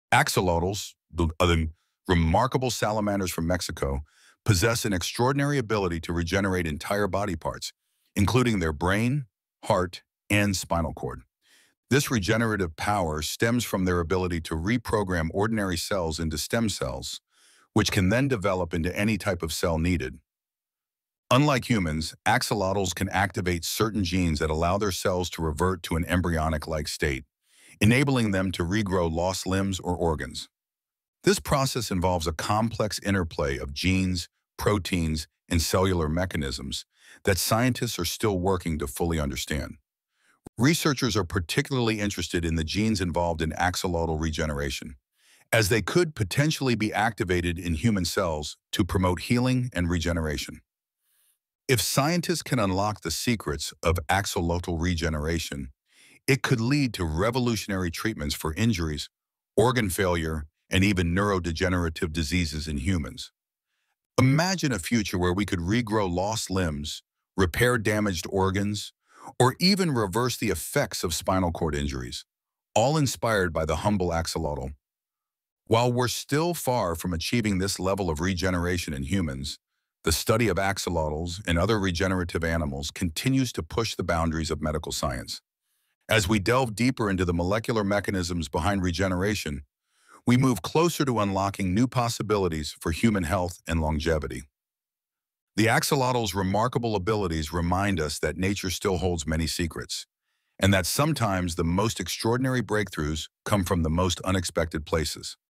Unlocking Nature's Secrets: The Incredible Regeneration of Axolotls 🌊✨
Dive into the mesmerizing world of axolotls, the amazing salamanders that can regenerate lost limbs, organs, and even parts of their brain! Join us as we explore their unique biology, the science behind their regenerative powers, and how this knowledge could revolutionize medicine for humans. 🌱❤️ Discover the fascinating connection between these incredible creatures and the future of healing!

👩‍🔬  Learn how researchers are unlocking the potential of regeneration for injuries and diseases, all inspired by the humble axolotl.